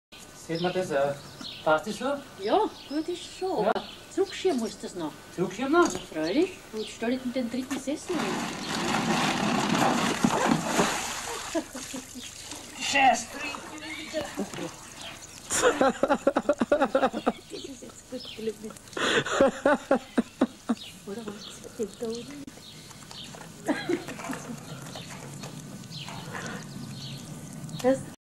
The Flower Pot
This man shifts the flower pot in a funny way.

Mann verschiebt Blumentopf.